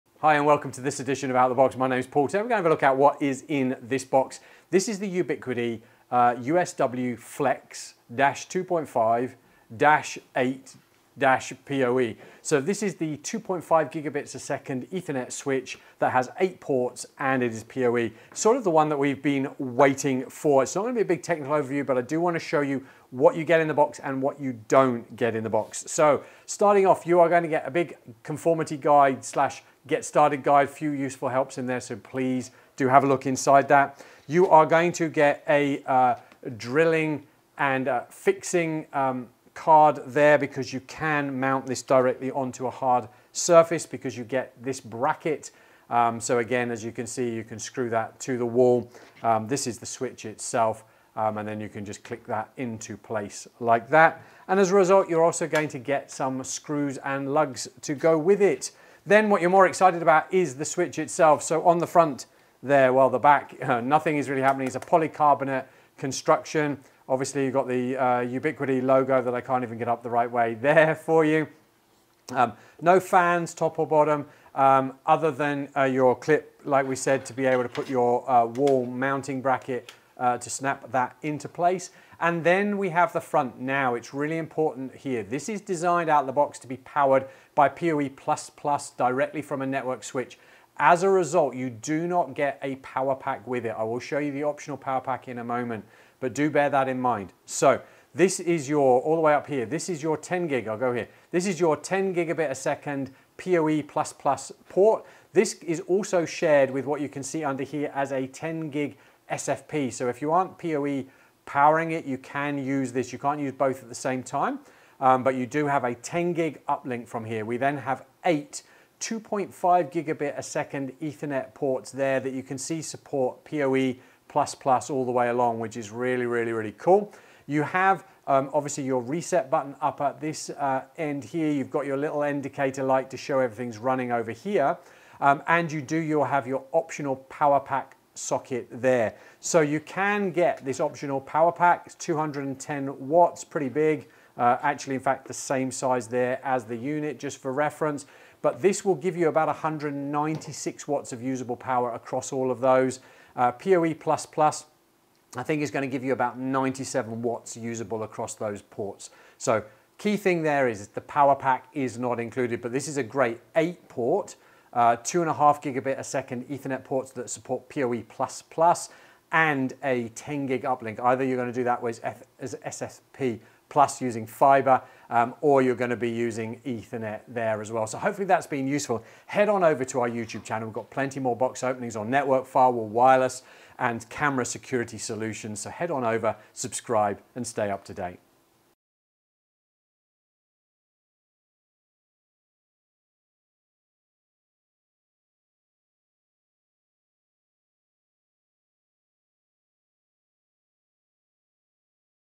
Out the Box Series - Ubiquiti USW-Flex-2.5G-8-PoE
This short, few-minute video gives you a quick overview of what you can find in the box when you receive your Ubiquiti USW-Flex-2.5G-8-PoE

The UniFi Switch Flex is a flexible, 8-port 2.5 GbE PoE++ switch for with a 10 GbE RJ45/SFP+ combination uplink port that can be powered with PoE+++ or an AC power adapter